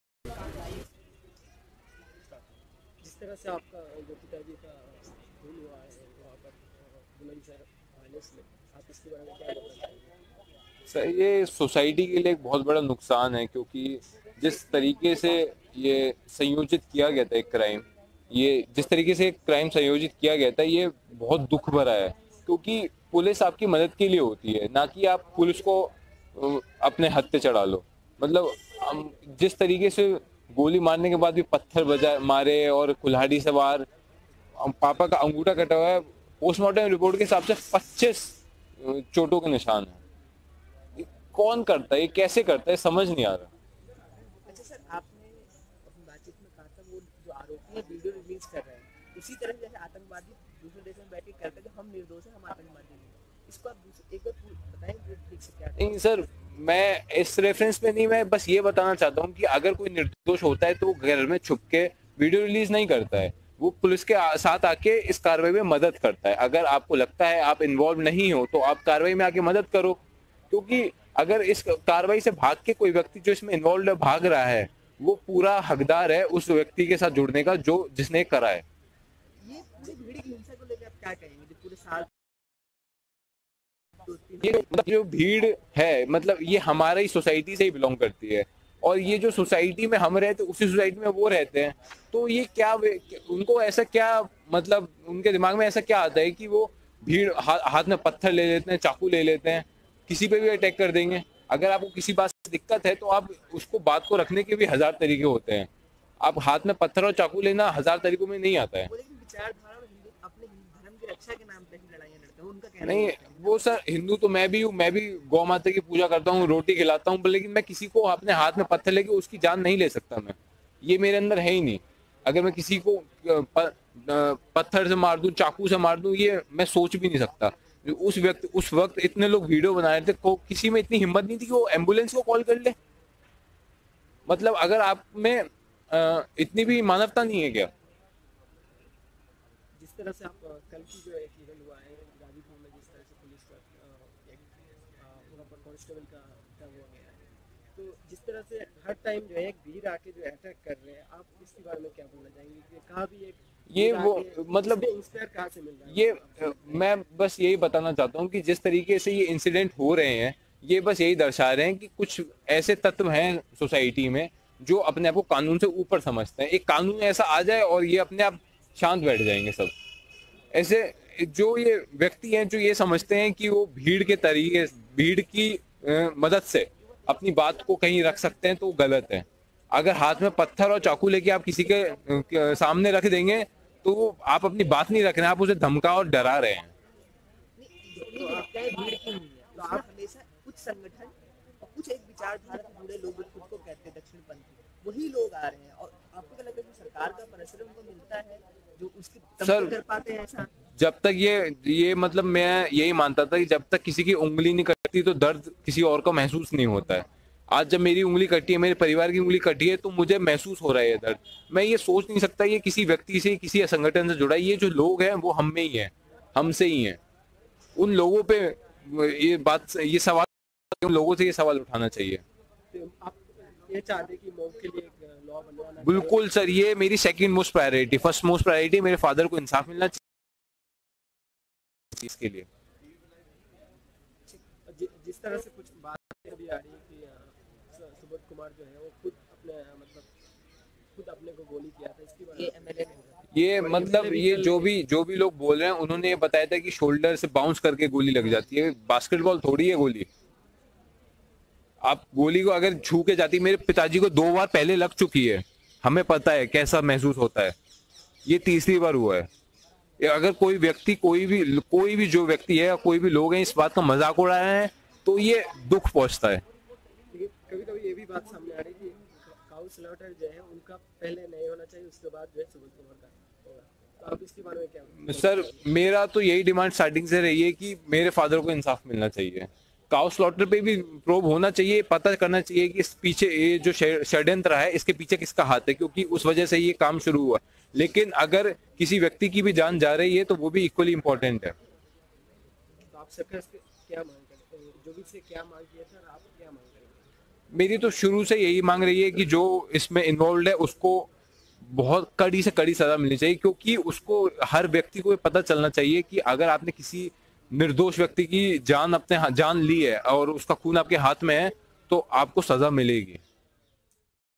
My first priority is Justice for my father :Inspector Subodh kumar's Son
"My first priority is Justice for my father and second most a law on mob lynching", said son of inspector Subodh Kumar.